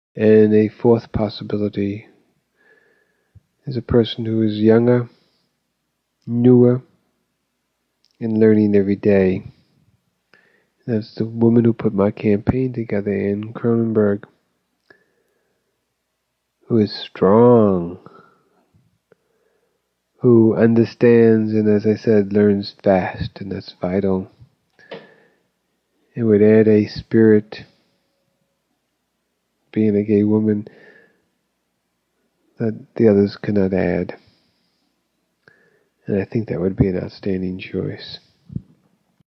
Harvey's 4th choice for his successor, Anne Kronenberg. [clip]
Harvey's 4th choice for his successor, Anne Kronenberg, was the choice many people in the gay community rallied behind—including Harry Britt. Why didn't Mayor Feinstein appoint her?May 7 on Queer Serial.
📸 Anne Kronenberg & Harvey Milk celebrate Election Night, 1977. Photo by Lance Wong.
📸 Anne & her motorcycle. Photo by Joyce Garay.
🤔 A couple photos of Kronenberg here I can’t find info about. Tips are appreciated. 💖